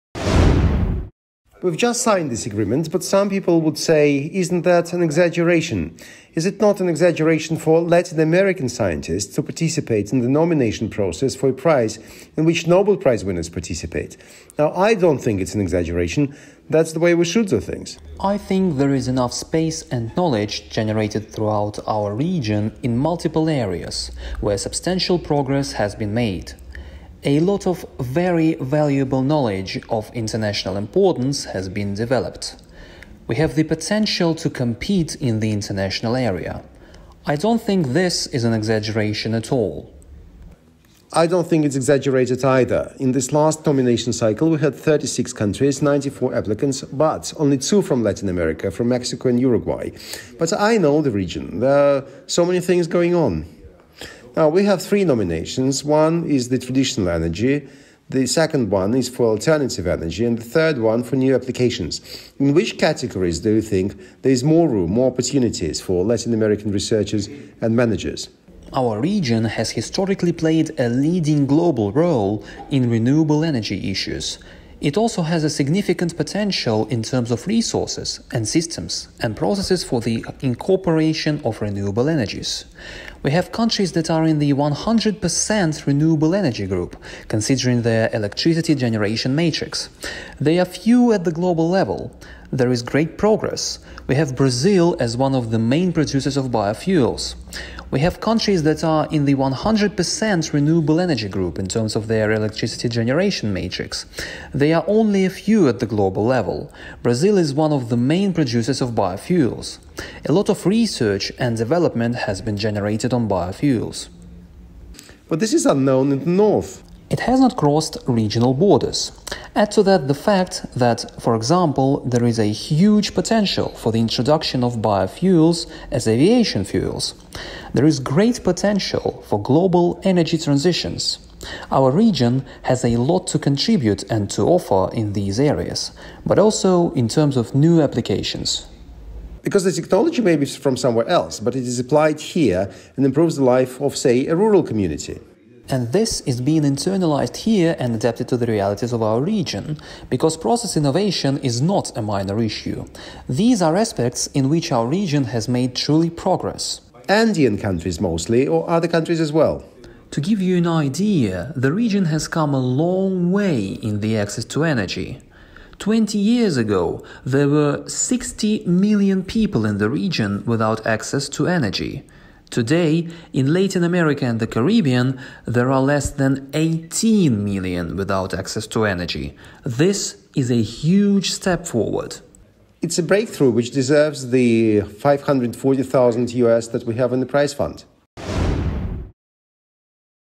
Interview with Executive Secretary of OLADE Alfonso Blanco
The call for applications for the Global Energy Prize opens on 1st December! Ahead of the event, we would like to share our interview with Alfonso Blanco, Executive Secretary of the Latin American Energy Organization (OLADE). We talked about energy development in Latin America and the involvement of Latin American scientists in the Prize's new nomination cycle.